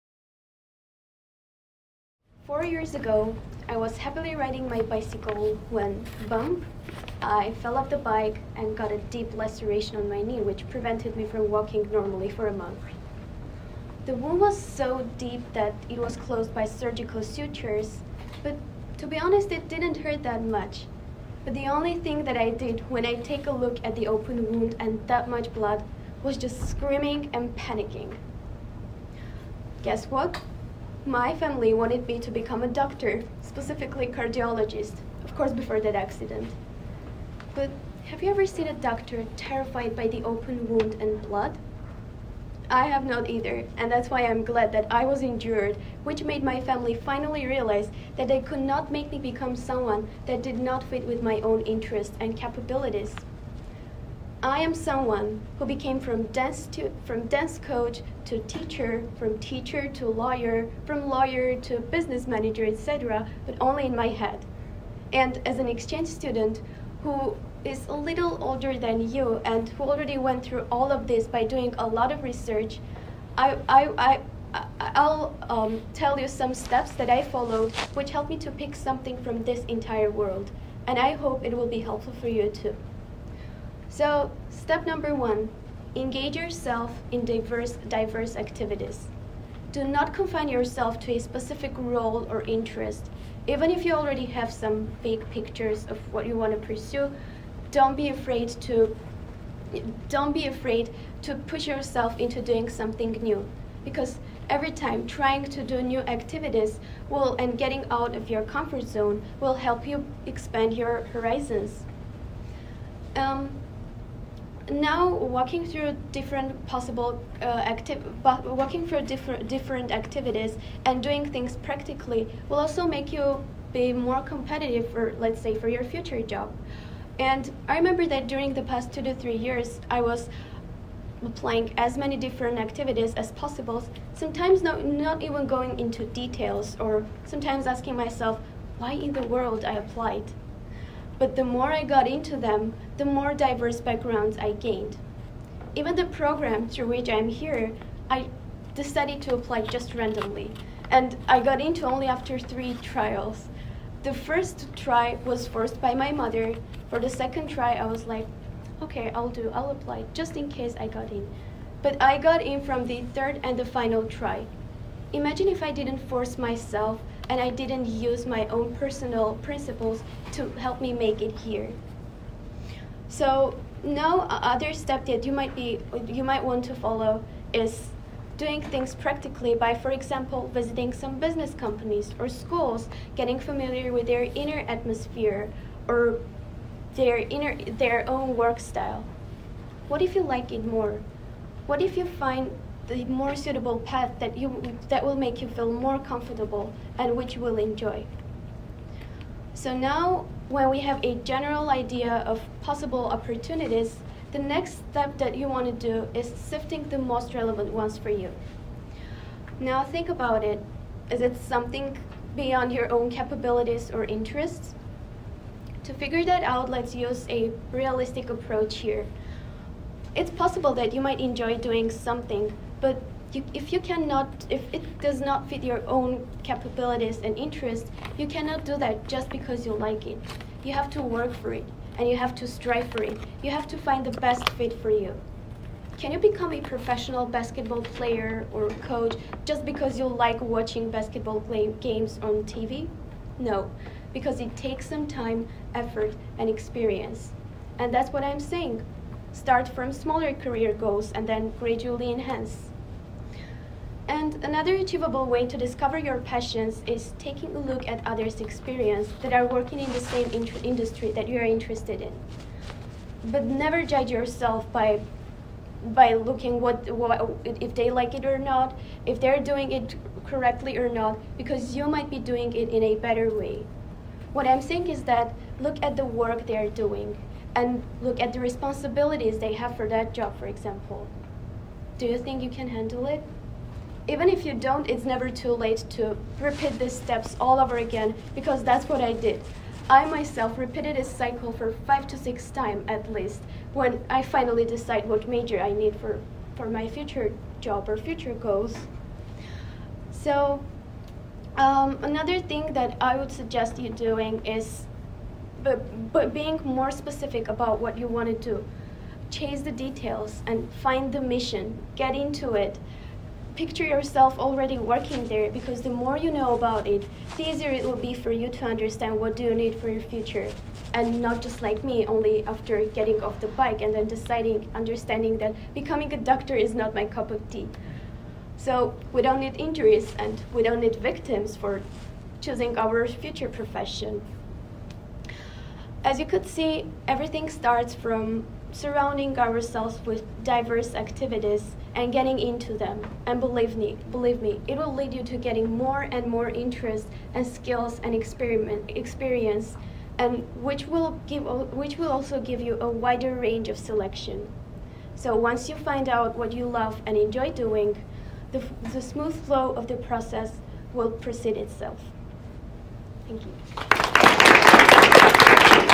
In Search of Your Own "You" | Milena Gharakeshishyan | TEDxShakerHS
Milena Gharakeshishyan advises on how to discover your passion and apply it to a career path.  Milena Gharakeshishyan is an exchange student from Armenia and a senior at Shaker. Already a high school graduate in Armenia, Milena will study Business at the American University of Armenia following her time at Shaker. She loves dealing with numbers, statistics and accounting. This talk was given at a TEDx event using the TED conference format but independently organized by a local community.